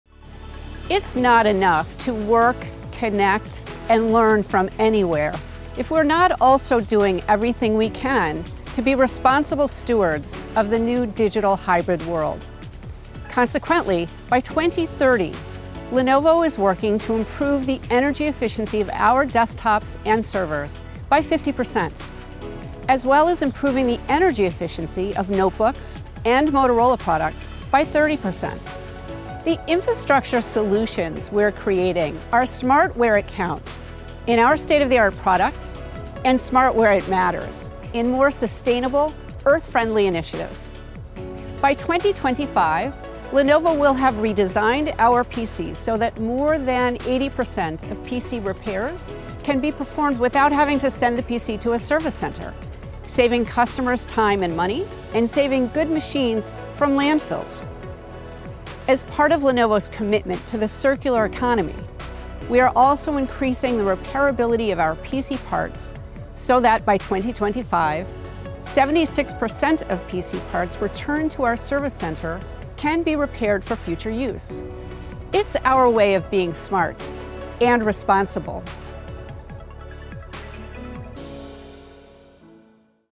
Lenovo Tech World 2021: Energy Efficient and Earth Friendly Initiatives
Lenovo is committed to making life better, society more inclusive & equitable, and the environment more sustainable.

What else do we have planned for a more sustainable future?